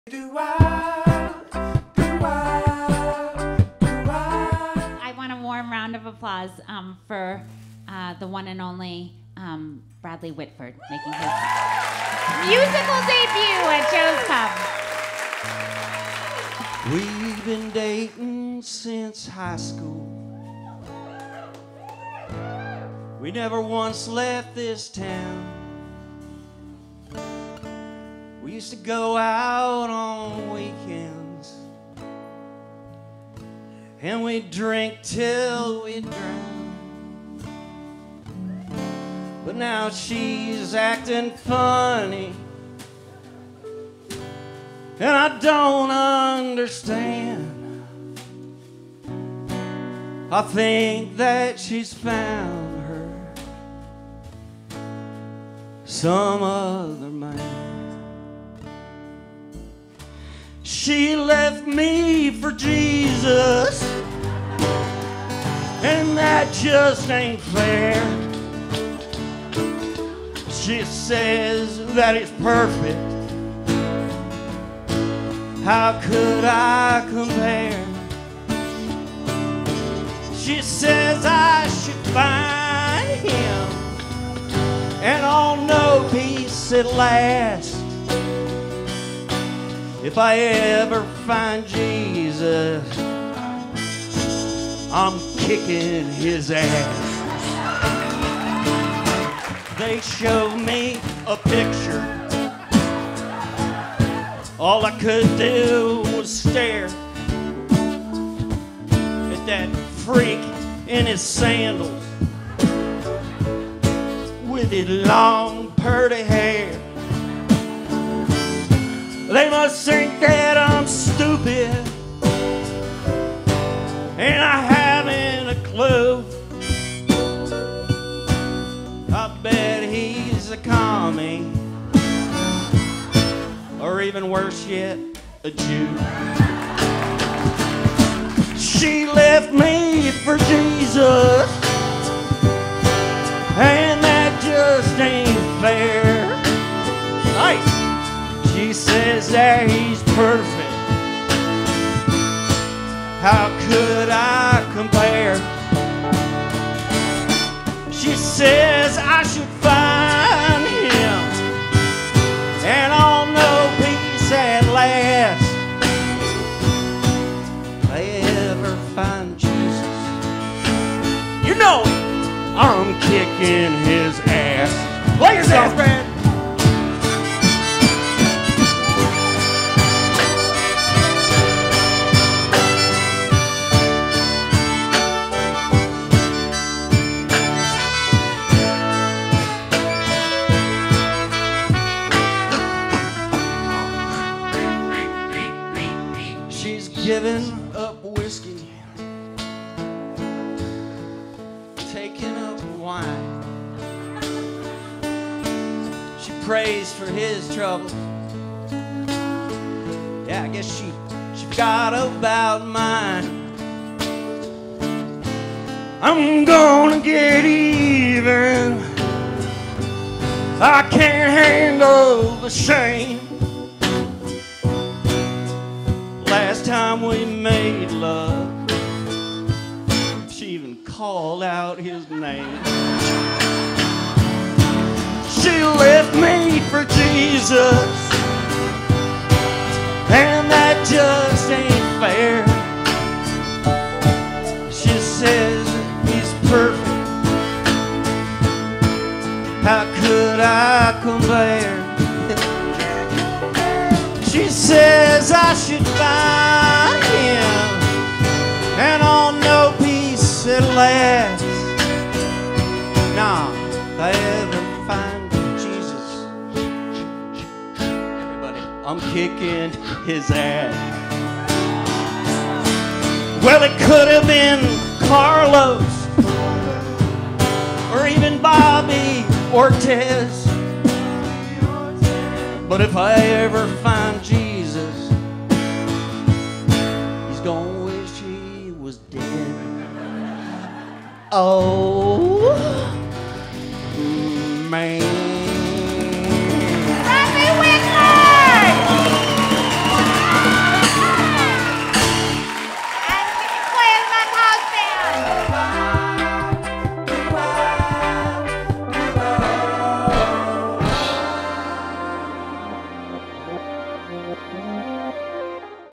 Get Out & The West Wing's Bradley Whitford Sings on EOTM
To hear their interview in full on iTunes or Soundcloud, go or iTunes for the podcast!

Recorded May 2017 at Joe's Pub | Video by Adam Abel and Meredith Kaufman Younger | Edited by Meredith Kaufman Younger | Graphics by Ed Mundy | Theme Music by Eric Biondo Band featuring MC Jelly D. and Shockwave, both of Freestyle Love Supreme.